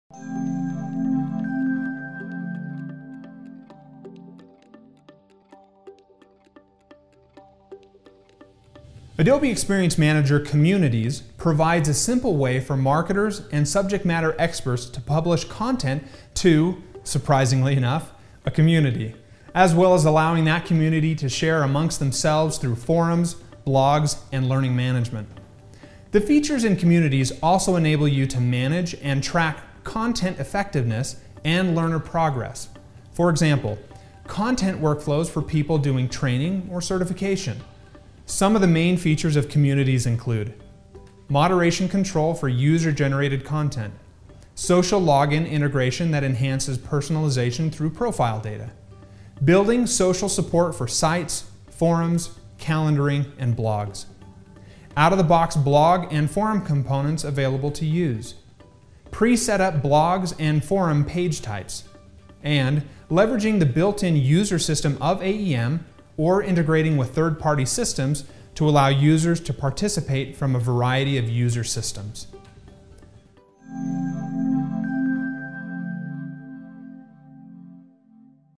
A video look at AEM and its Features: Communities
There is often a lot of confusion about what AEM is.  Watch Peter Nash, technical project manager at Axis41, explain the features that make up Adobe Experience Manager Communities.